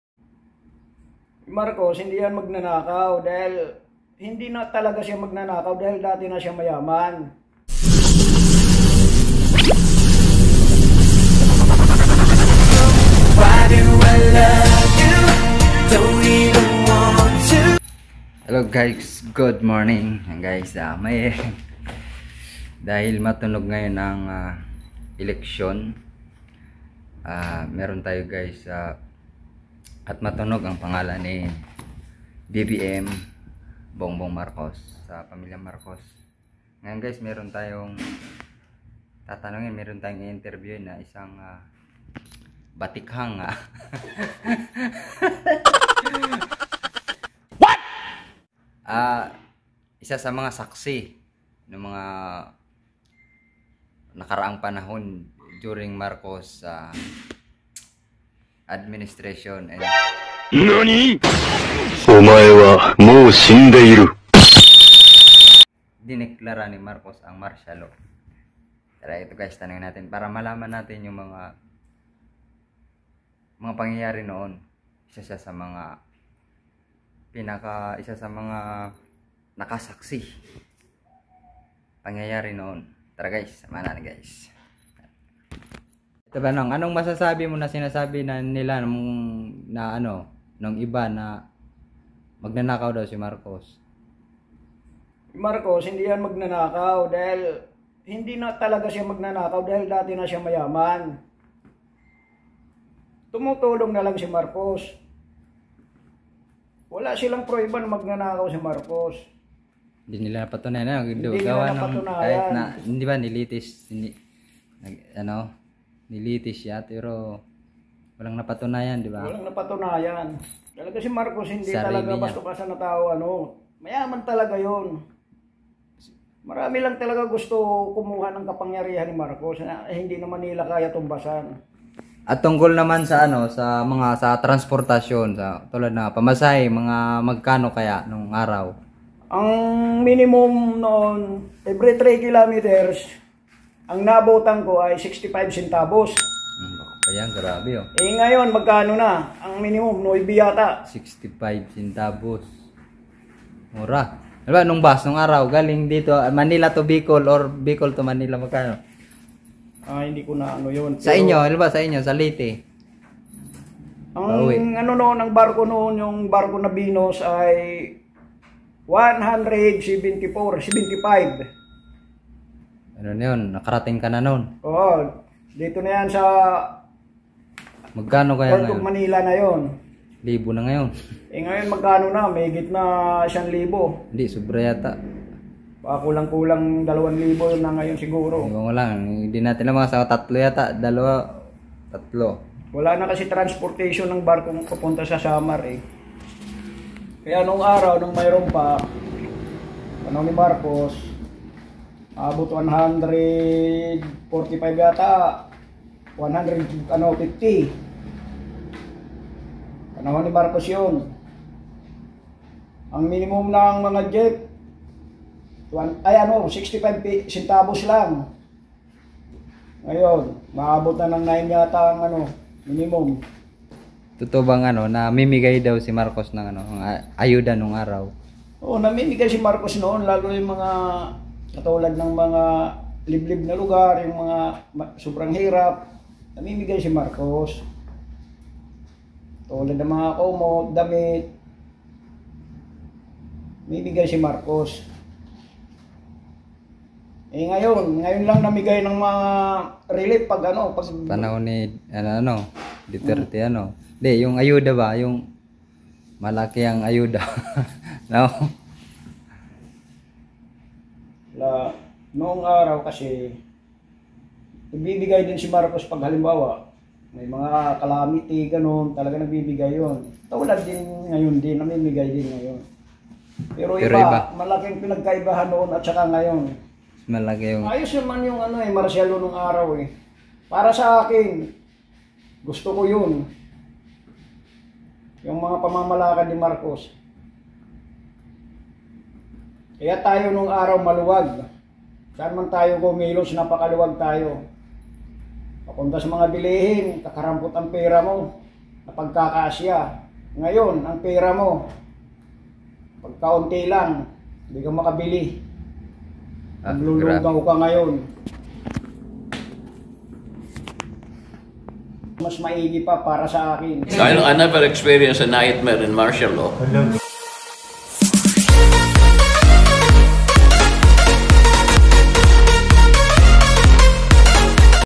Grabi panahon pala noon panahon ng Marcial Law/marcial law/ferdinand marcos/manong rey@valesticktv
Ang videong ito ay naglalaman ng dagdag kaalaman ng nagdaang mga taon. .